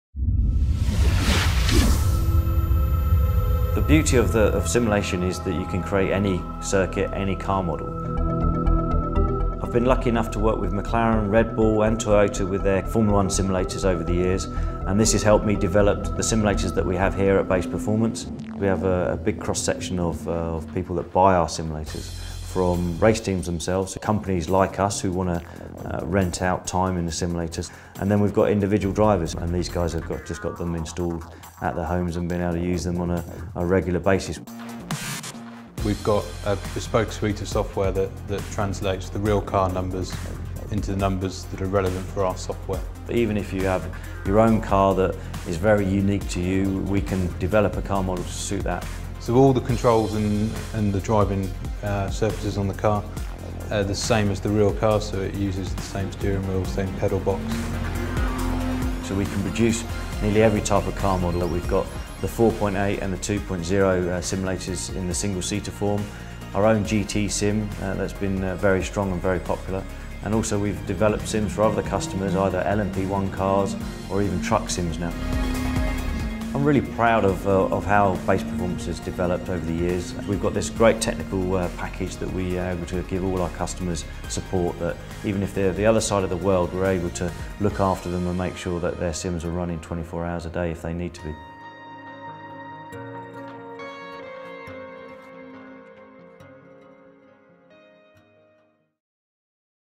Base Performance Simulators - Available From AWE
Now Distributed by AWE!

Base Performance Simulators (BPS) is one of the UK’s leading race simulation companies, led by three-time Le Mans winning Aston Martin factory driver, Darren Turner.

BPS can be purchased from channel partner AWE Europe.